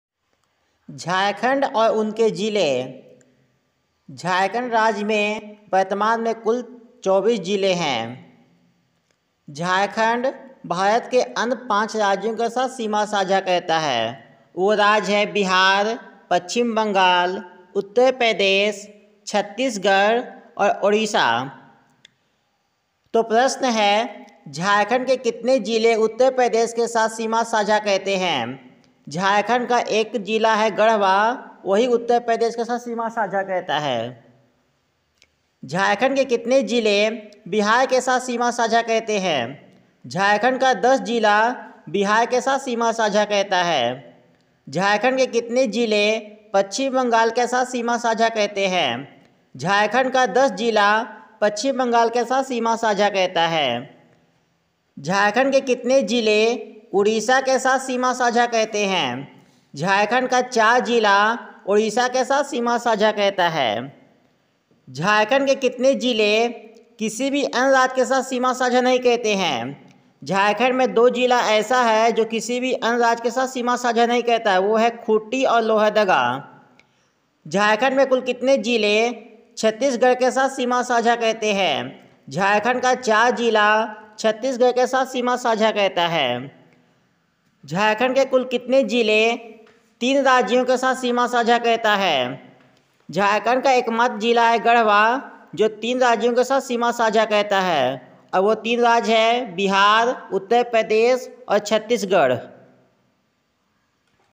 झारखंड और उनके जिले jharkhand and it's district
Jharkhand rajya mein kitne jile Hain
Jharkhand rajya Bharat ke kitne rajyon ke sath Seema sajha karta hai
how many states share boundary with Jharkhand